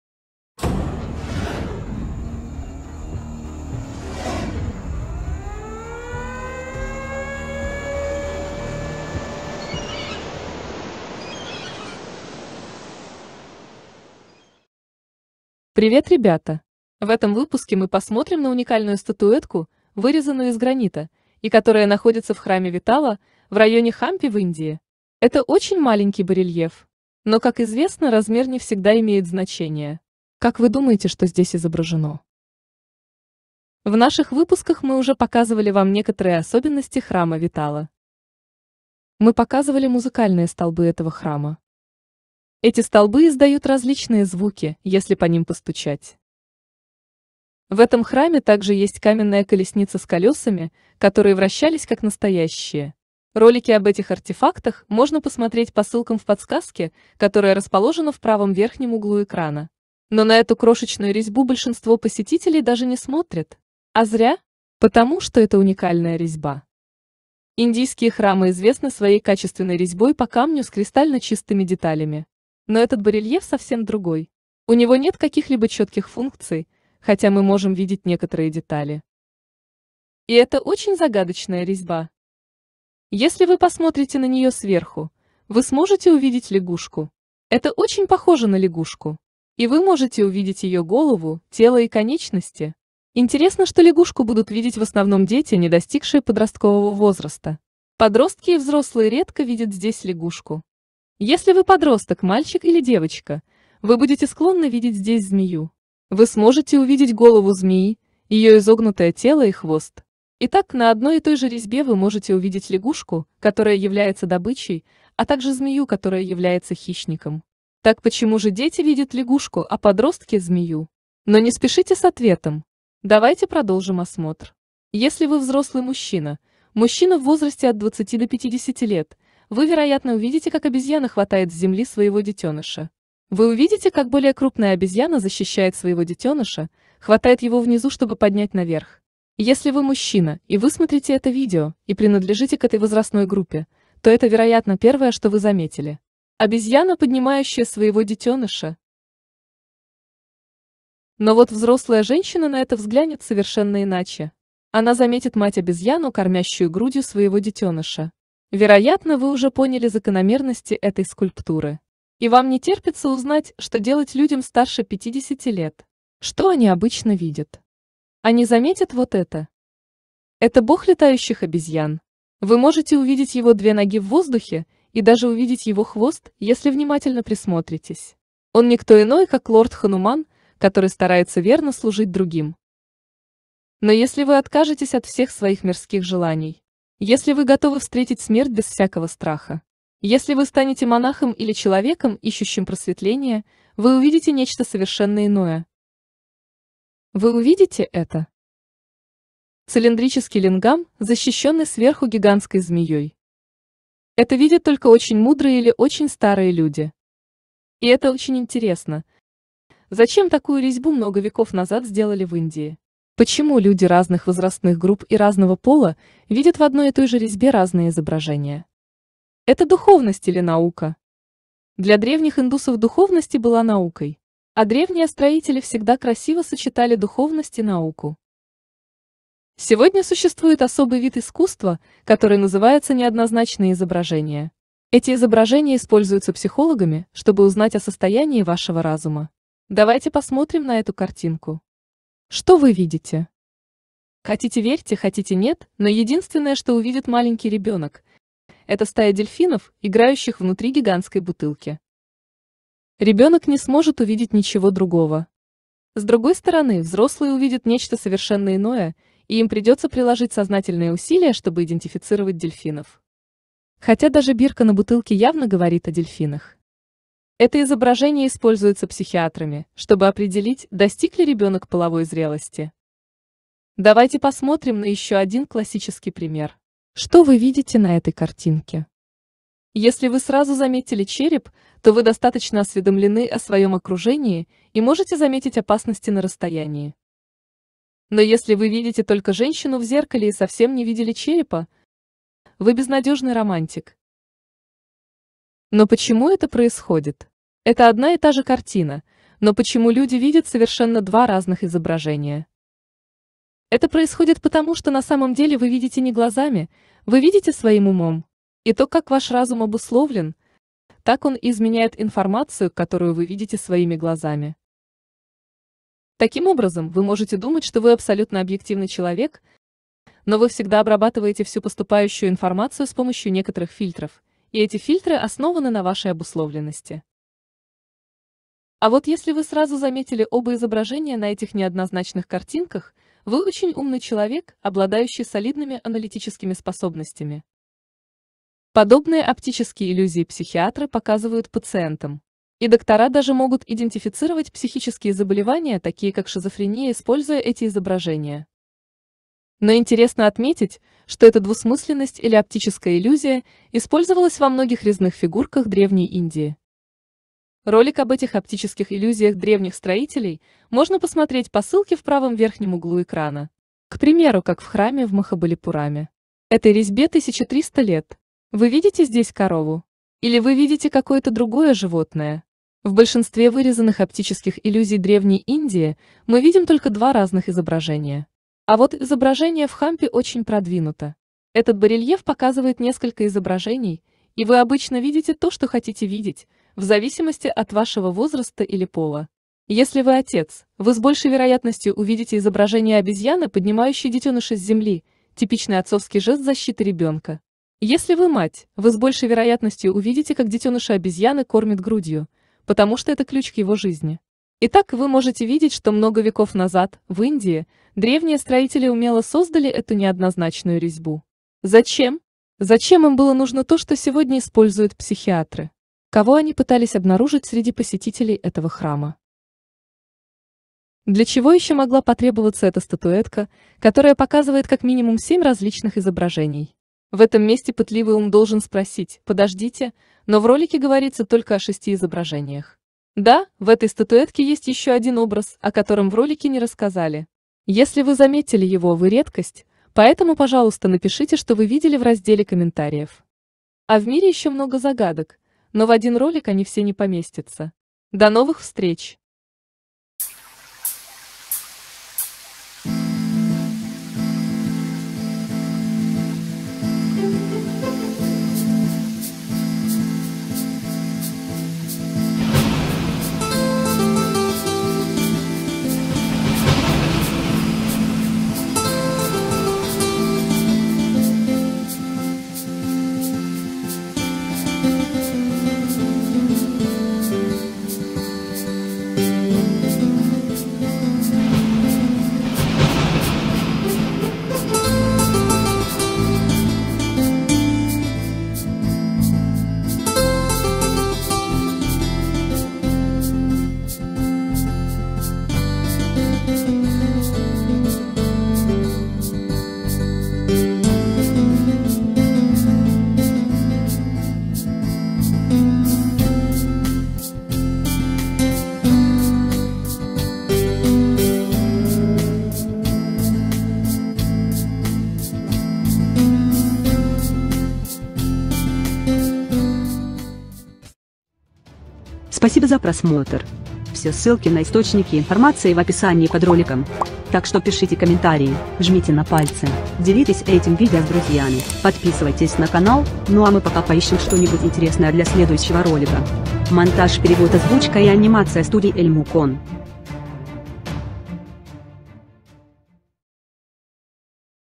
Скульптура 7 в 1. Психологи из древнего мира? [№ B-028.12.08.2020.]
Привет ребята. В этом выпуске мы посмотрим на уникальную статуэтку, вырезанную из гранита, и которая находится в храме Виттала в районе Хампи в Индии. Это очень маленький барельеф. Но, как известно, размер не всегда имеет значение. Как вы думаете, что здесь изображено? В наших выпусках мы уже показывали вам некоторые особенности храма Виттала. Мы показывали музыкальные столбы этого храма. Эти столбы издают различные звуки, если по ним постучать. В этом храме также есть каменная колесница с колёсами, которые вращались как настоящие. Но на эту крошечную резьбу большинство посетителей даже не смотрят. А зря. Потому, что это уникальная резьба. Индийские храмы известны своей качественной резьбой по камню с кристально чистыми деталями. Но этот барельеф совсем другой. У него нет каких-либо четких функций, хотя мы можем видеть некоторые детали. И это очень загадочная резьба. Если вы посмотрите на неё сверху, вы сможете увидеть лягушку. Это очень похоже на лягушку. И вы можете увидеть ее голову, тело и конечности. Интересно, что лягушку будут видеть в основном дети, не достигшие подросткового возраста. Подростки и взрослые редко видят здесь лягушку. Если вы подросток, мальчик или девочка, вы будете склонны видеть здесь змею. Вы сможете увидеть голову змеи, ее изогнутое тело и хвост. Итак, на одной и той же резьбе вы можете увидеть лягушку, которая является добычей, а также змею, которая является хищником. Так почему же дети видят лягушку, а подростки - змею? Но не спешите с ответом. Давайте продолжим осмотр...
Источник - канал PraveenMohan
EL.MU.CON.
это сокращение от
Electronic Multi Configuratur
по русски - НАСТРОЙЩИК "РОЯЛЕЙ".